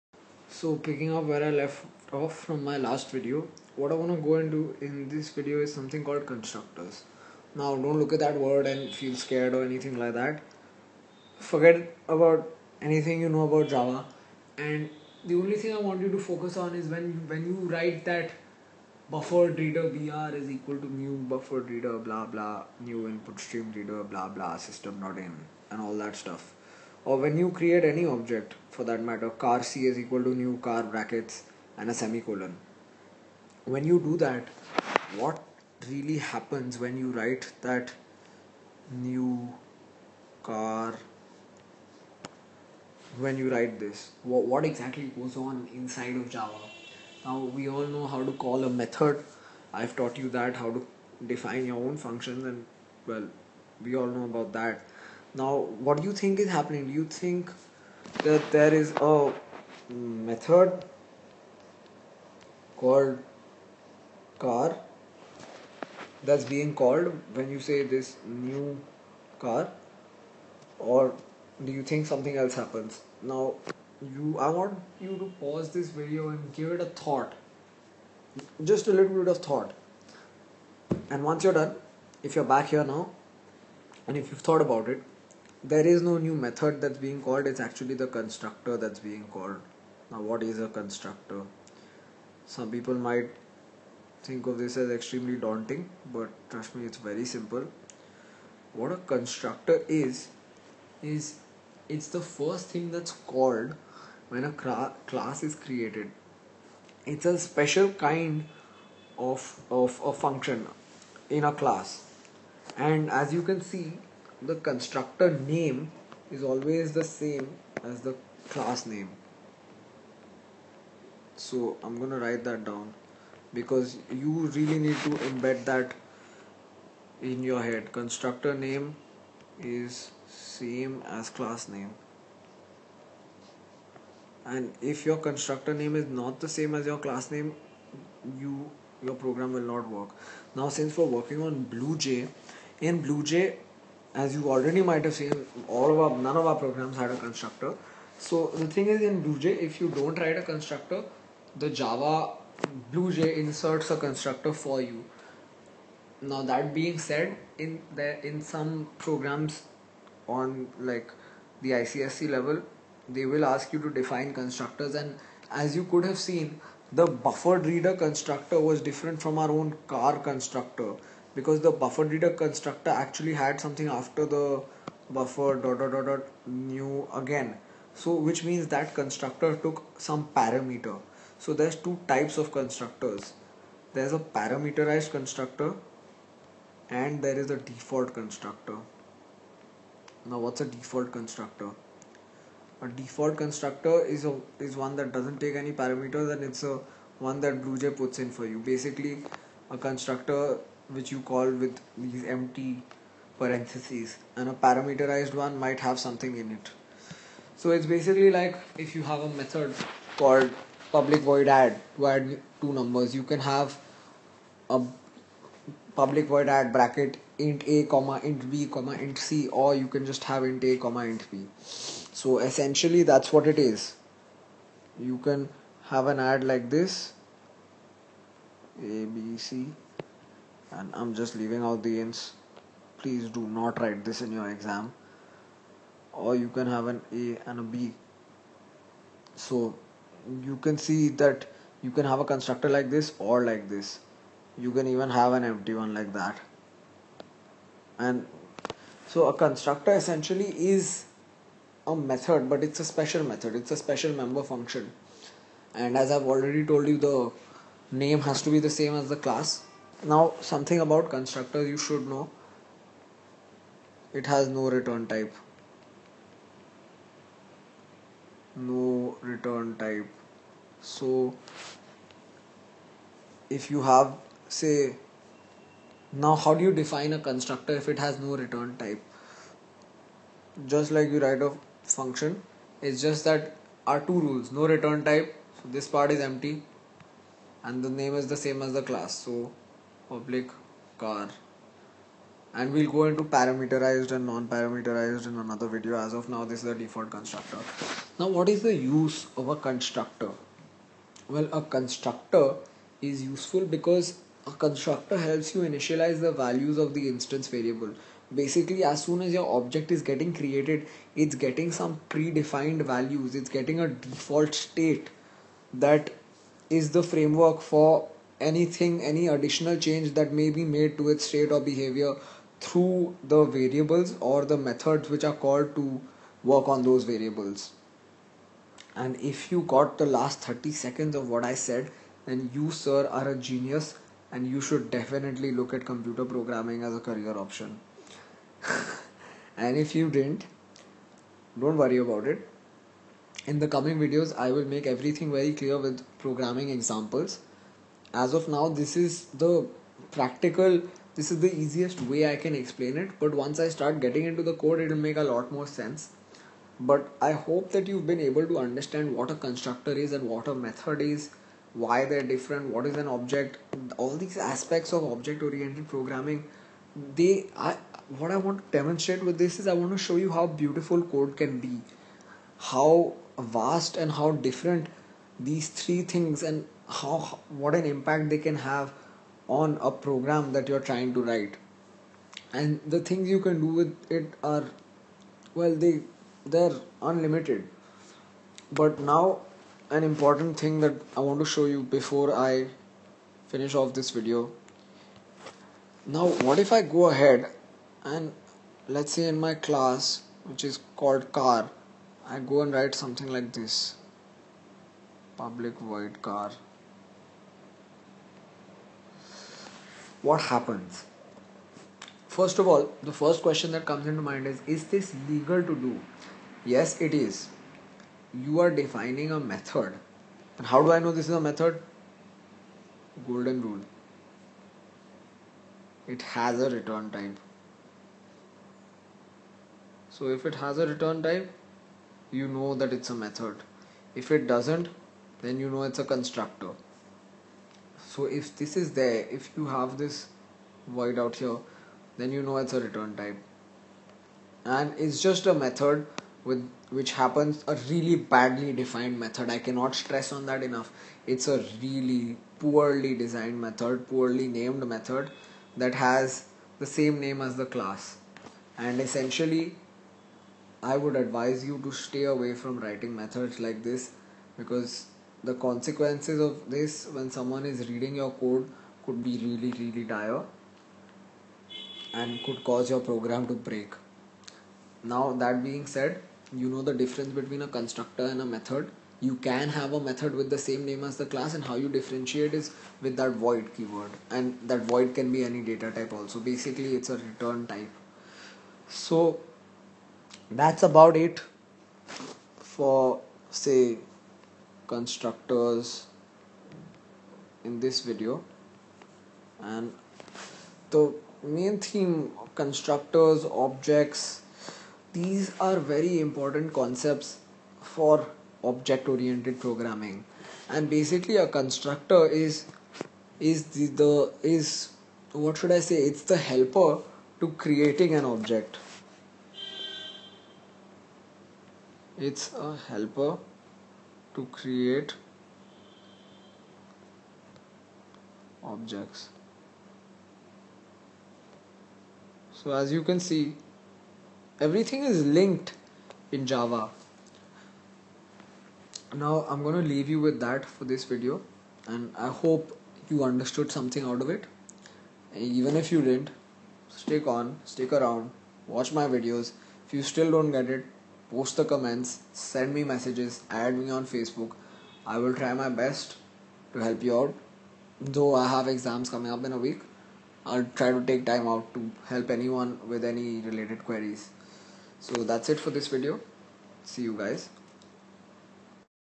ICSE Java Programming using blueJ : #30. Constructors in Java
An introduction to how objects are instantiated using constructors in Java.